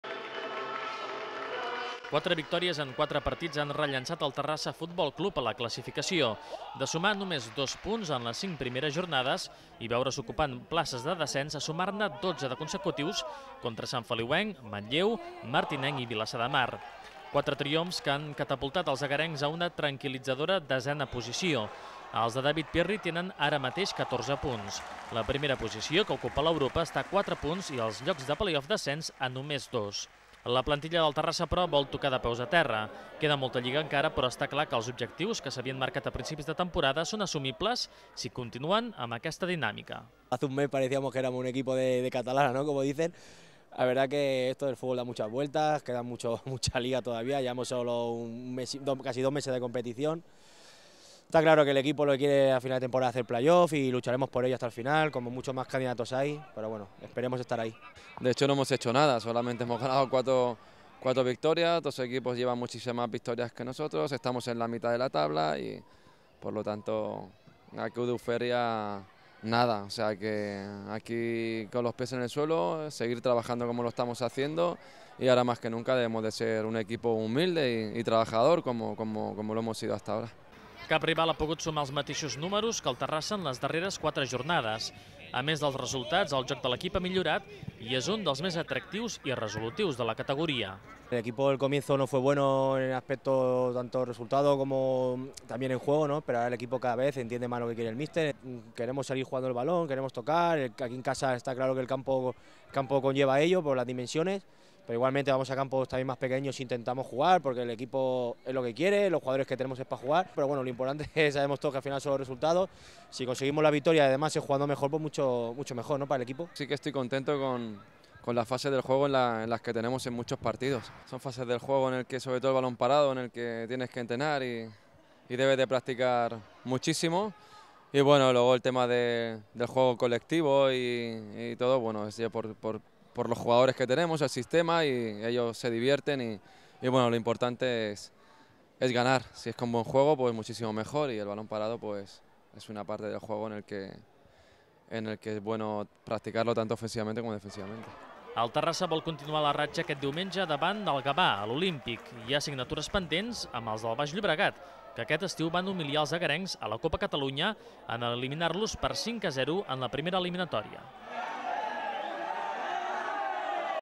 El Terrassa FC presenta candidatura amb números de play-off d'ascens
Quatre victòries en quatre partits han rellançat els de David Pirri. De sumar només dos punts en les cinc primeres jornades i veure’s ocupant places de descens, a sumar-ne 12 de consecutius.